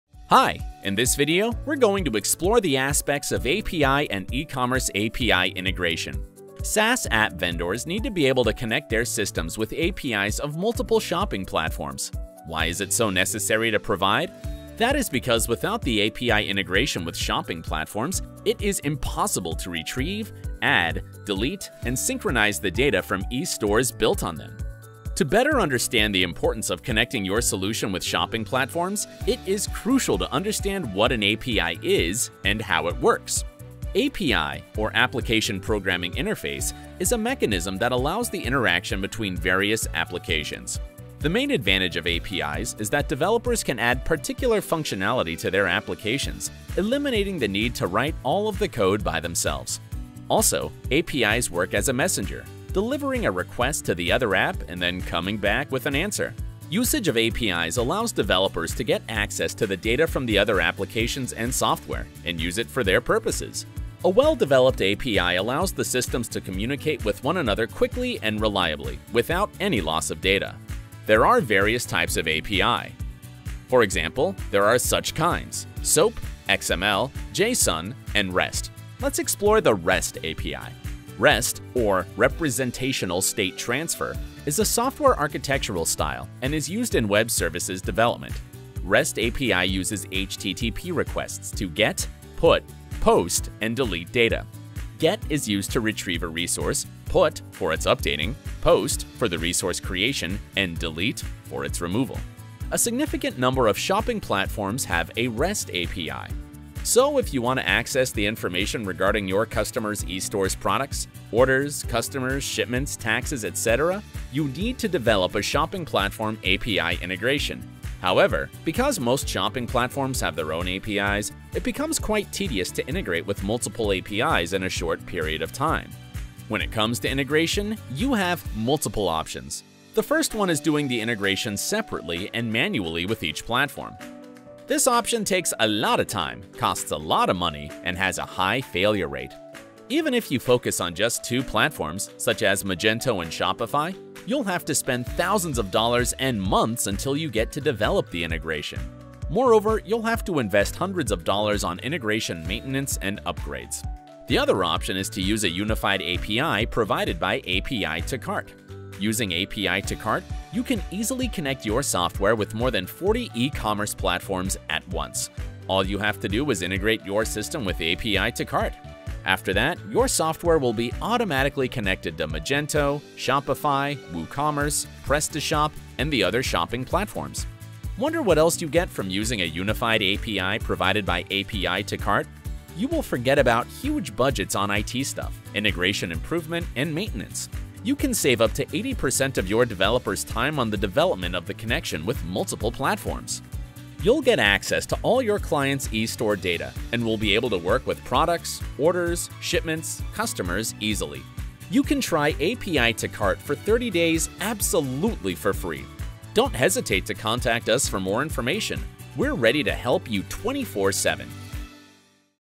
What is API and API integration? | API2Cart
Explore the aspects of API and eCommerce API integration.

API or Application Programming Interface is a mechanism that allows the interaction between various applications.

The main advantage of APIs is that developers can add particular functionality to their applications eliminating the need to write all of the code by themselves. Also, APIs work as a messenger, delivering a request to the other app and then coming back with an answer.

One of the most popular types of API is REST API. It uses HTTP requests to GET, PUT, POST and DELETE data. GET is used to retrieve a resource, PUT - for its updating, POST - for the resource creation and DELETE - for its removal.

eCommerce API integration is the process of tying any application to shopping cart software. API integration with shopping platforms is highly essential as it allows retrieval, adding, deleting, and synchronizing the data from e-stores built on them.

The best option for developing a connection with multiple shopping platforms is to use API2Cart. Usage of API2Cart allows your SaaS solution to integrate with 60+ eCommerce platforms and marketplaces, including Magento, Shopify, OpenCart, PrestaShop, Amazon, eBay, etc. at once.